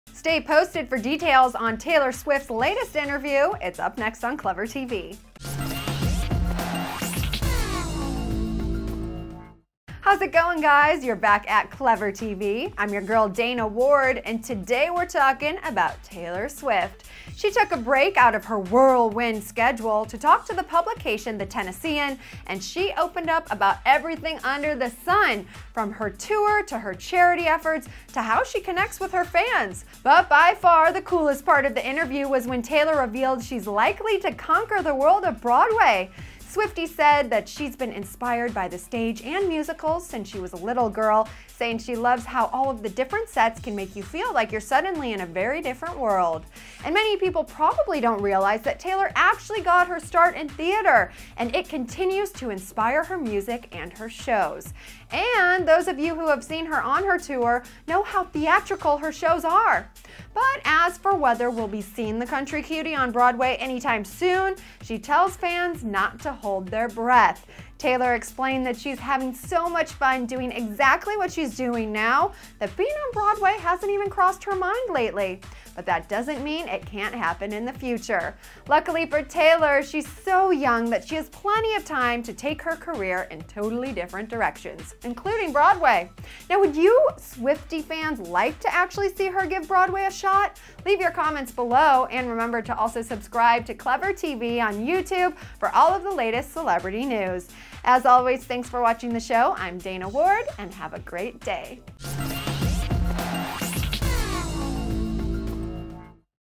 Taylor Swift Wants To Do Broadway Musical
Stay posted on details from Taylor Swift's latest interview. It's up next on ClevverTV!

How's it going guys, you're back at ClevverTV. I'm your girl Dana Ward, and today we're talking about Swifty-aka Taylor Swift. She took a break out of her whirlwind schedule to talk to the publication the Tennessean, and opened up about everything under the sun--from her tour, to her charity efforts, to how she connects with her fans. But by far the coolest part of the interview was when Taylor revealed she's like to conquer the world of Broadway!

Swifty says she's been inspired by the stage and musicals since she was a little girl, saying she loves how all the different sets can make you feel like you're suddenly in a different world. And many people probably don't realize that Taylor actually got her start in theater, and continues to inspire her music and her shows. And those of you who have seen her on tour know how theatrical her shows are! But as for whether we'll be seeing the country cutie on Broadway any time soon, she tells fans not to hold their breath. Taylor explained that she's having so much fun doing exactly what she's doing now, that being on Broadway hasnt crossed her mind lately. But that doesnt mean it can't happen in the future! Luckily for Taylor, she's so young that she has plenty of time to take her career in different directions. Would you Swifty fans like to see her give Broaday a shot? I'm Dana Ward, have a great day!